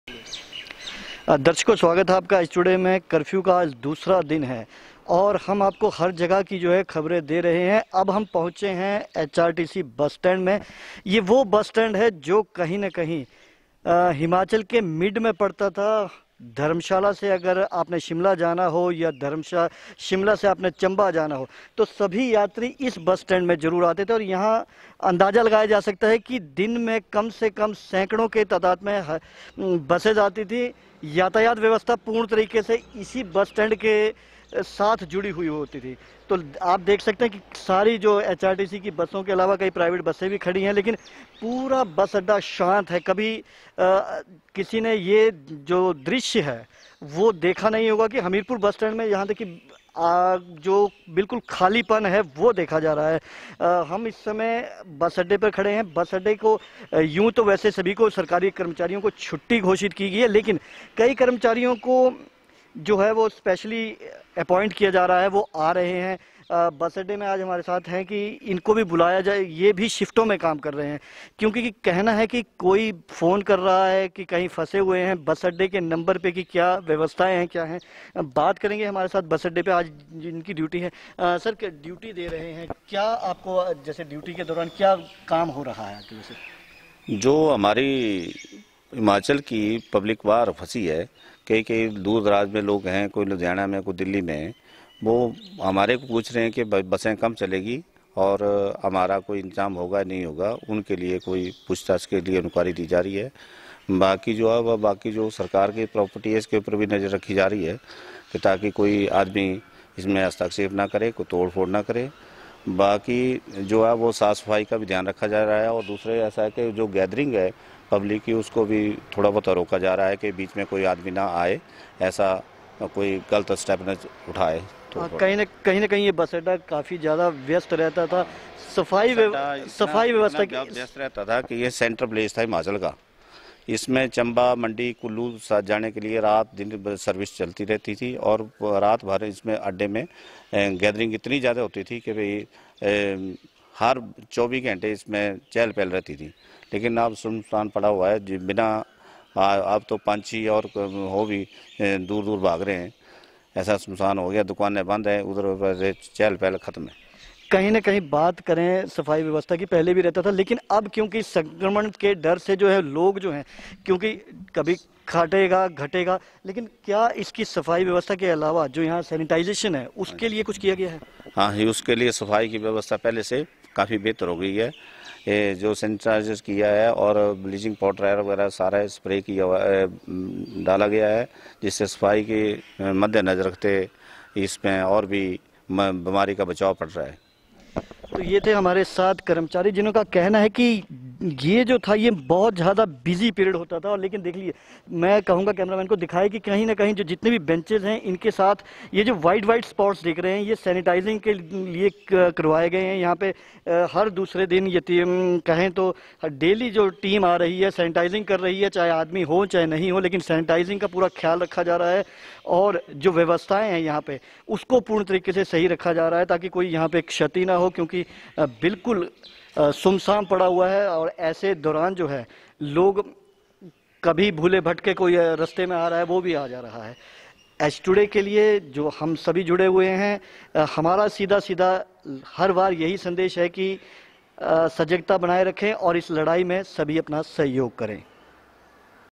curfew में हमीरपुर के सबसे व्यस्ततम बस अडडे पर पसरा सन्नाटा, देखिए लाइव रिपोर्ट
दिल्ली, चडीगढ, शिमला, मनाली के लिए कभी होती यात्रियों के लिए मारोमार , curfew में हमीरपुर के सबसे व्यस्ततम बस अडडे पर पसरा सन्नाटा, देखिए लाइव रिपोर्ट